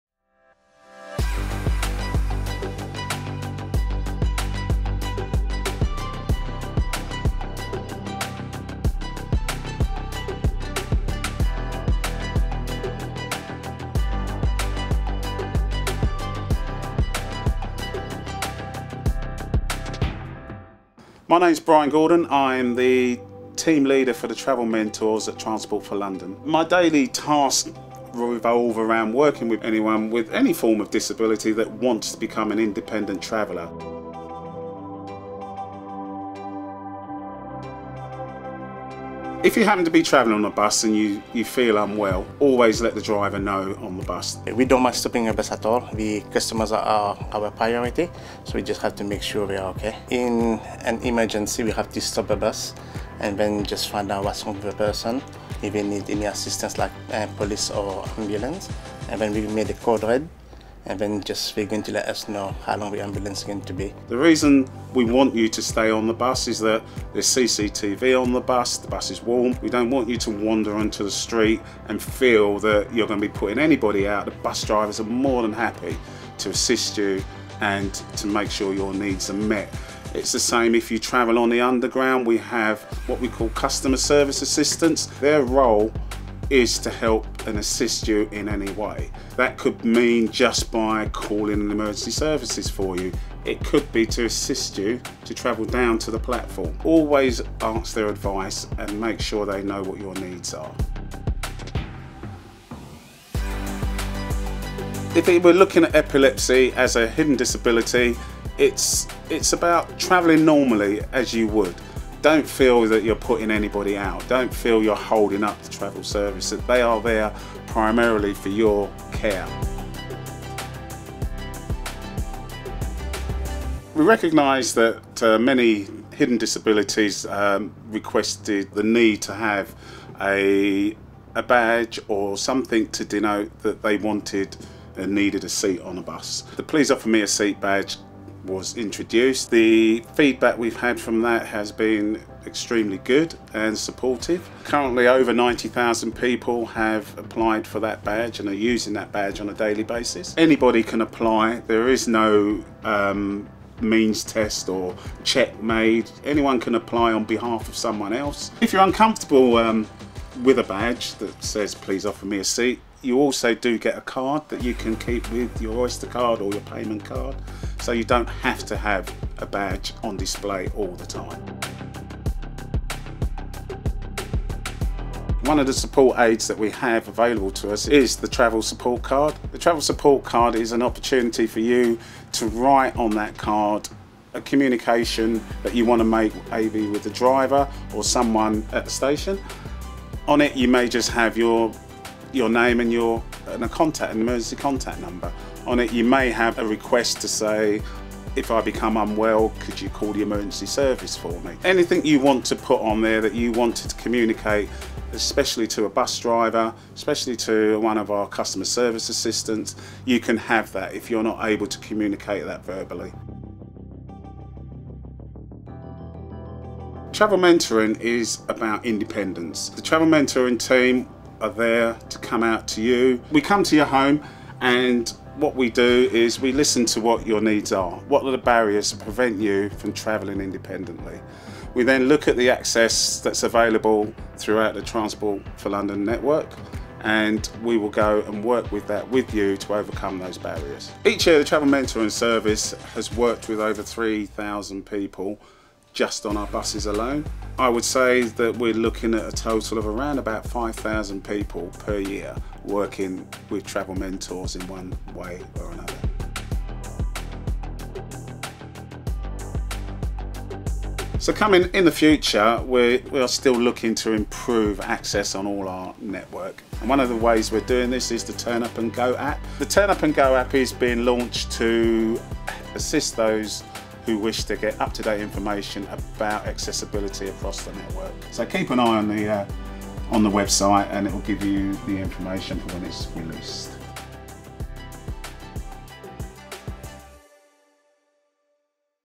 Feeling Safe on Public Transport
Young Epilepsy met with Transport for London to find out how people with hidden disabilities like epilepsy can travel safely, and what initiatives are in place to support them.